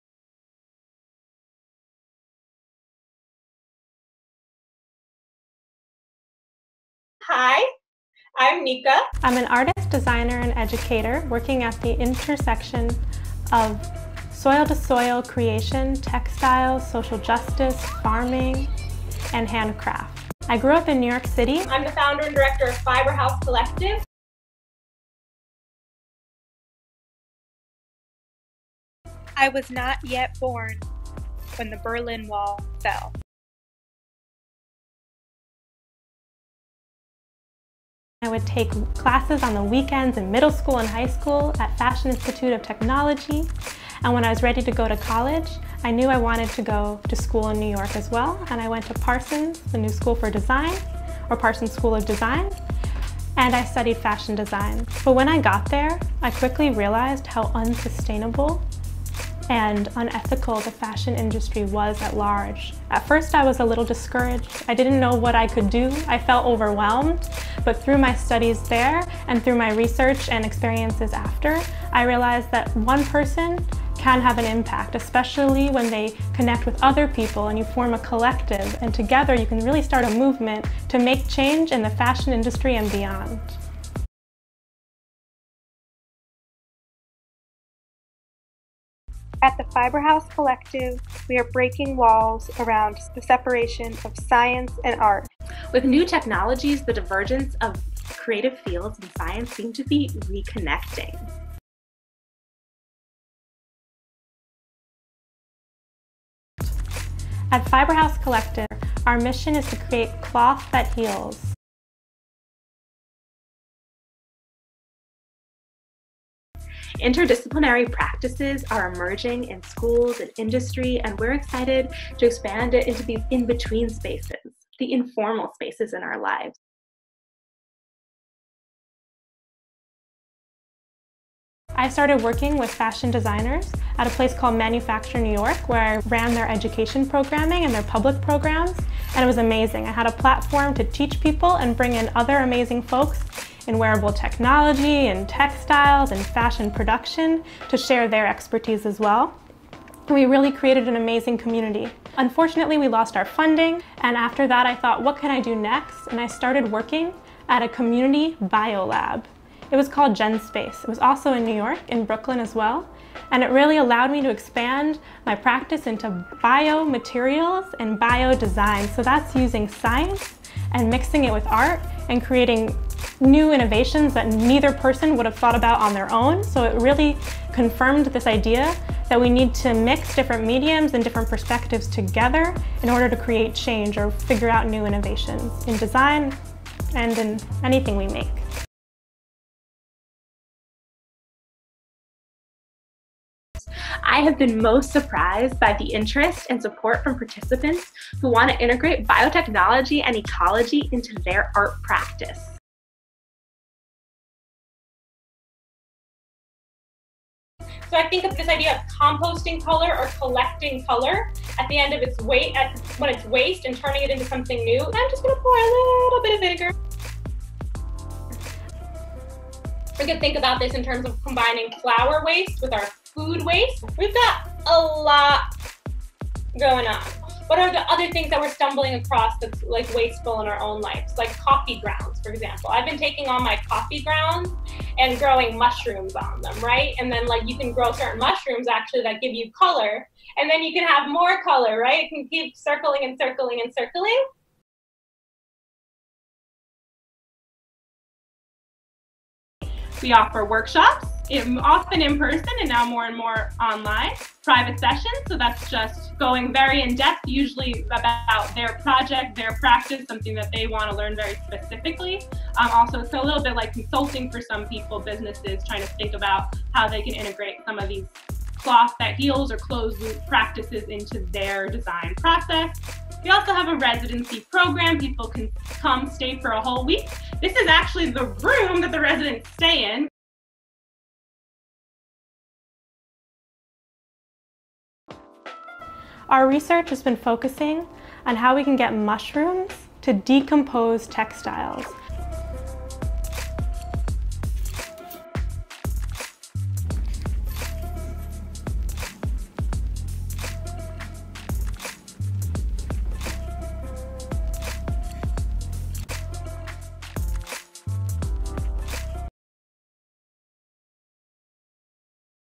Breaking the Wall between Science and Art
Nica Rabinowitz  is a Falling Walls Finalist at the Falling Walls and Berlin Science Week: World Science Summit 2020 (1 – 10 November).
Will she become the Breakthrough of her category Science Engagement Initiatives? Register for free and don't miss the remote event